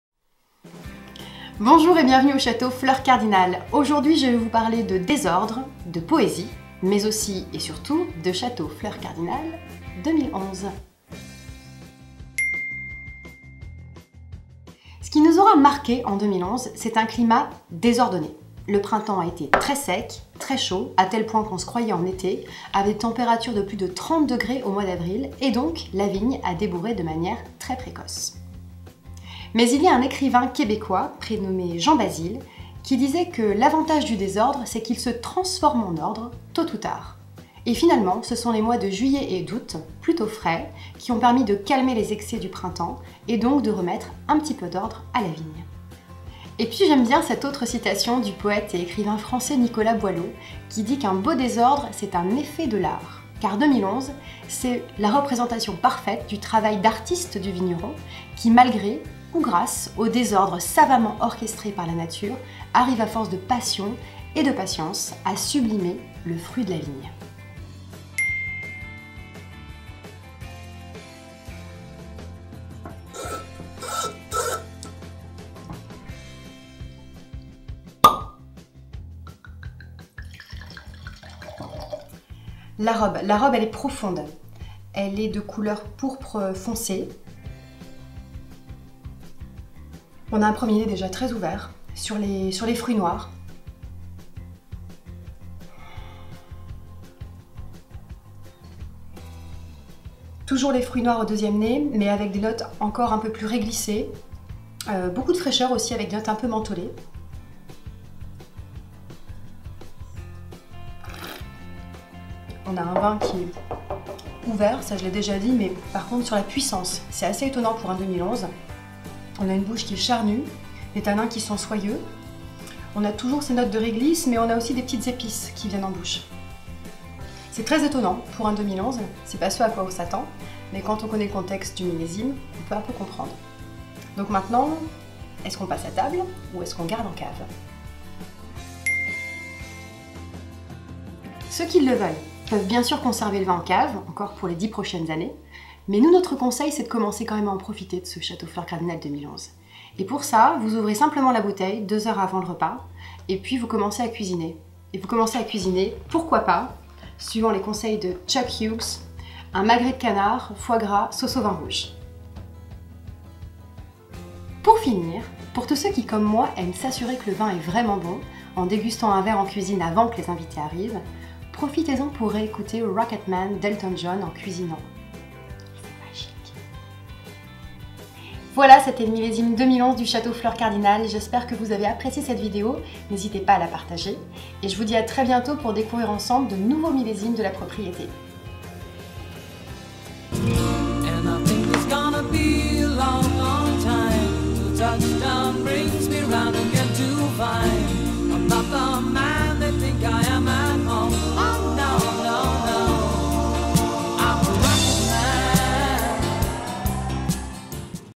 MILL. 2011 / LA DEGUSTATION // THE TASTING
Voici le premier épisode d’une série consacrée aux millésimes de Château Fleur Cardinale. Nous avons souhaité ces vidéos comme des éléments de réponse pour tous les amateurs de nos vins qui se posent les questions suivantes : en quelques mots, comment s’est passé le travail à la vigne cette année-là ? Comment se présente le millésime aujourd’hui ? Peut-on commencer à l’ouvrir ou faut-il encore attendre ? Quelles sont nos suggestions pour l’accompagner ?
Le tout raconté par nos soins, avec nos mots et nos envies du moment. Nous avons donc le plaisir de diffuser ce jour la vidéo du millésime 2011 !
Here is the first episode of a series about the vintages of Château Fleur Cardinale. We wanted these videos as answers for all the lovers of our wines, who may sometimes wonder: just in few words, how was the work in the vineyard in any particular year? How does the wine taste at the moment? Is it ready to drink or should you wait? What are our suggestions to go well with it?
Everything is told with our own words, in the mood of the moment. So today we are very glad to show you the video of the 2011 vintage!

Livre de recettes : Chuck Hughes - Garde-Manger - Editions La Presse